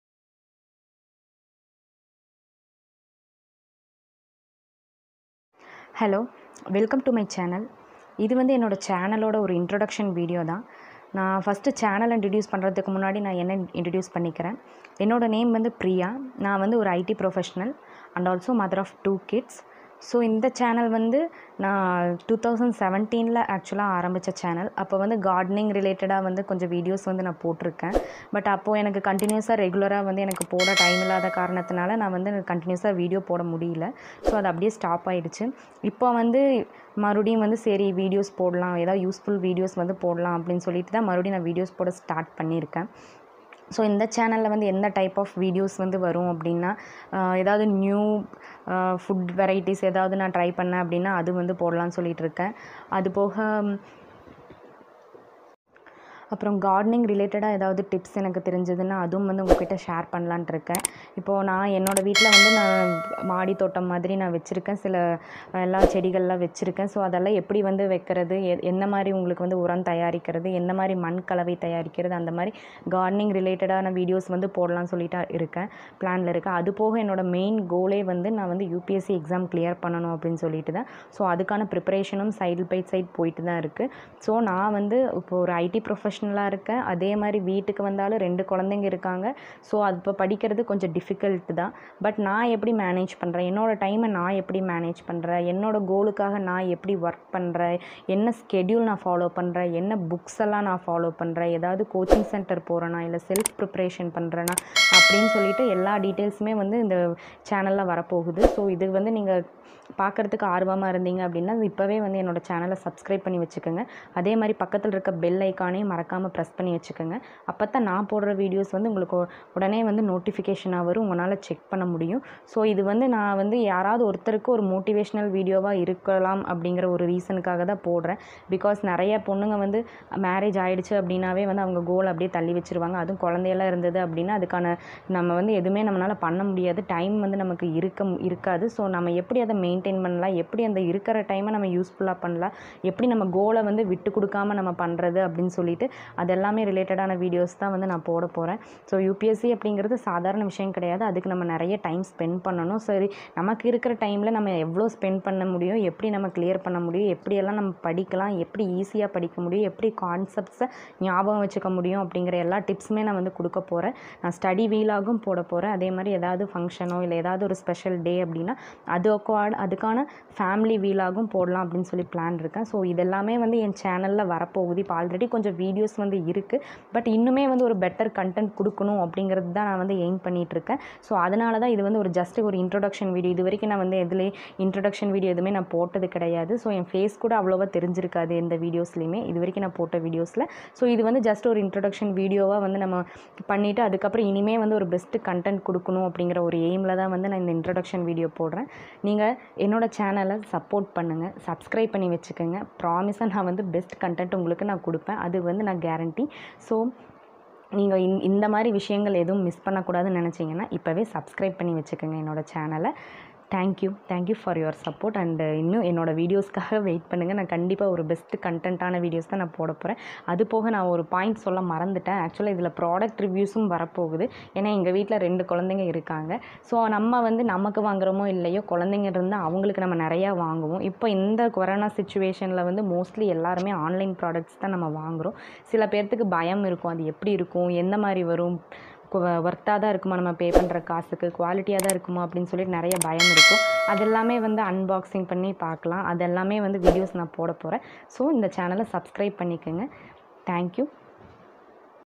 Introduction Video | Priya talkzz | Cooking | Gardening | Vlogs|UPSC Preparation.
This video explains about the purpose of this channel.

Cooking, Gardening, Study and Family vlogs, Upsc preparation Strategy

Study vlog
Family vlogs
Upsc preparation
Upsc preparation Strategy
UPSC BOOKS